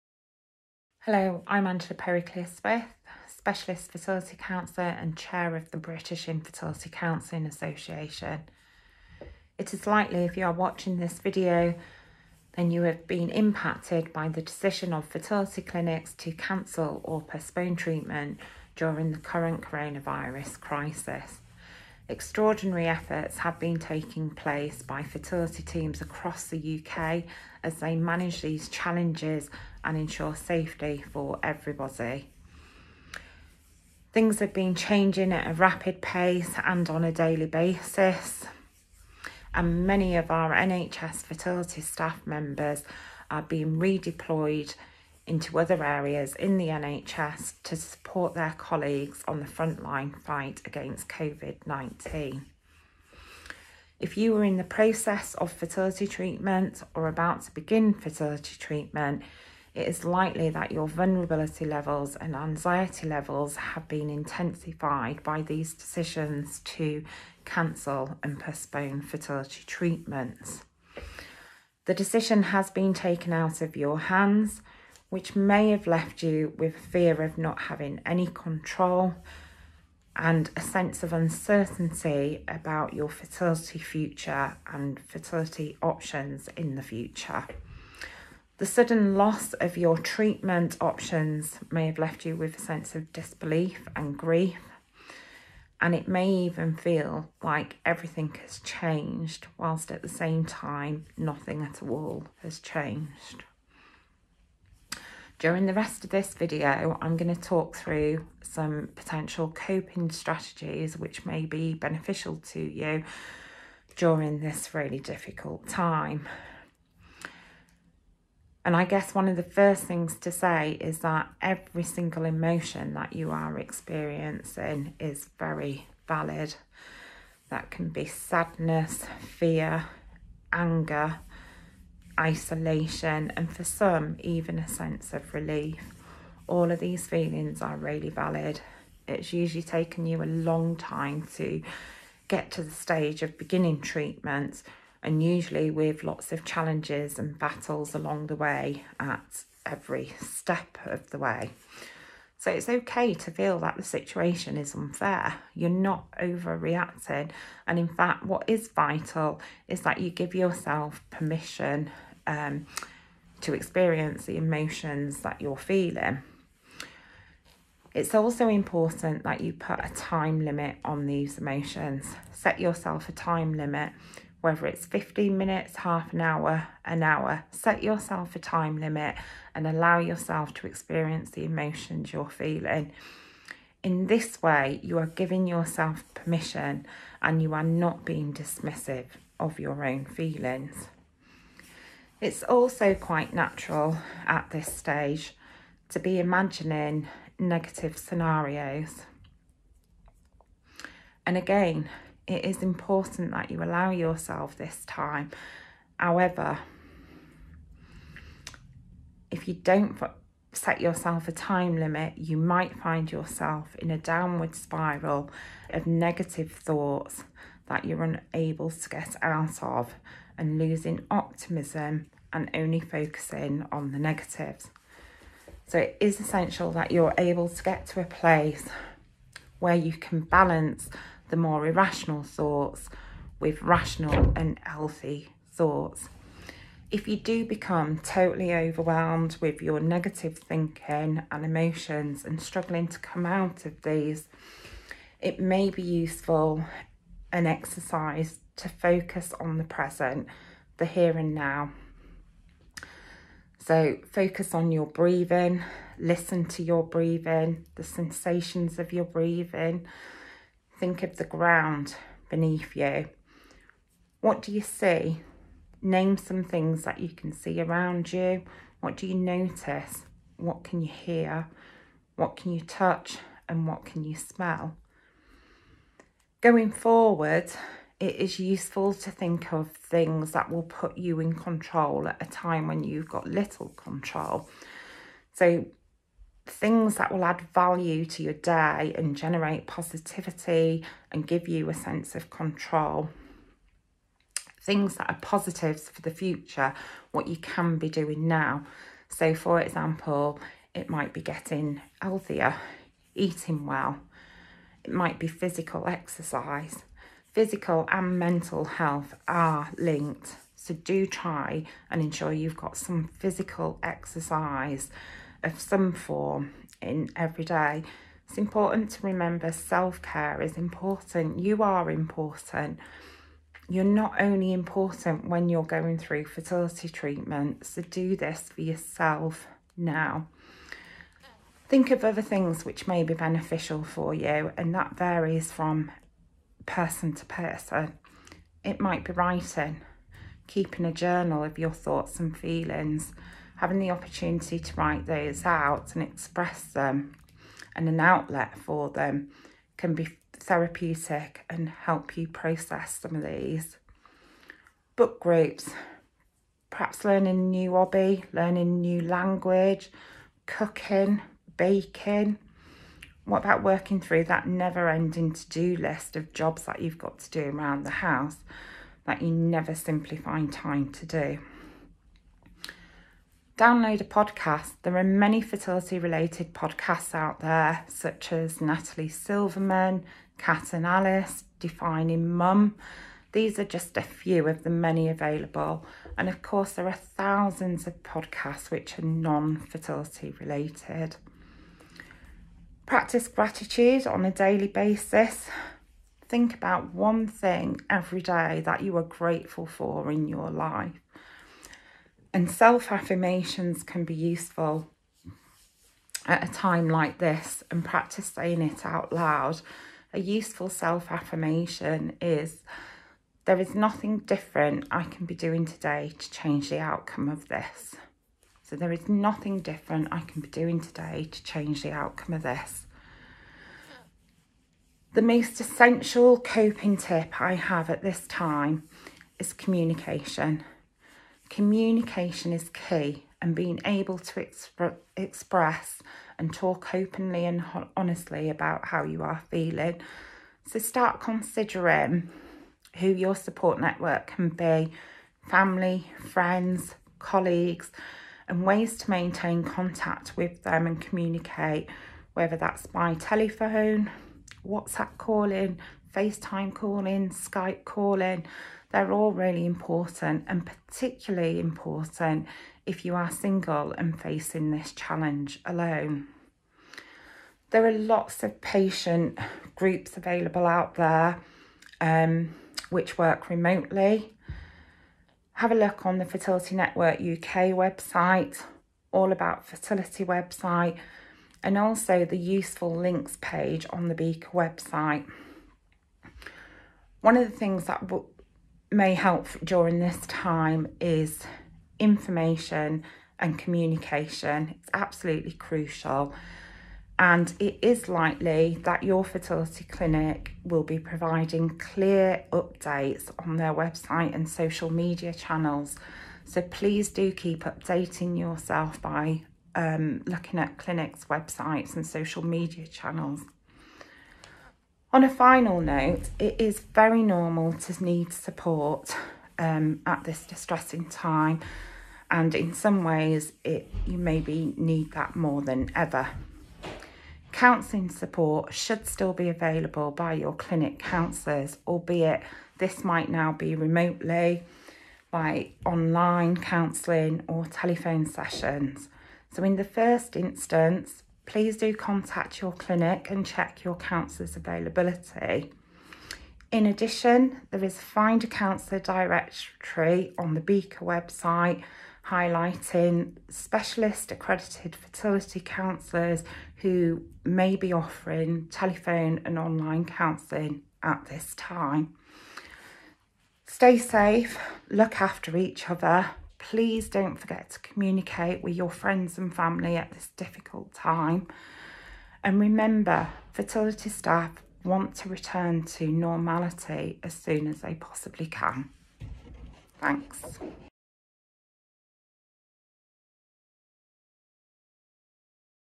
Fertility treatment and COVID-19
Angela Pericleous-Smith, a specialist fertility counsellor, talks about fertility treatment and the current situation with COVID-19. Watch this video to get an update on fertility treatment during COVID-19. The video is for you who have had your fertility treatment cancelled or postponed due to the pandemic.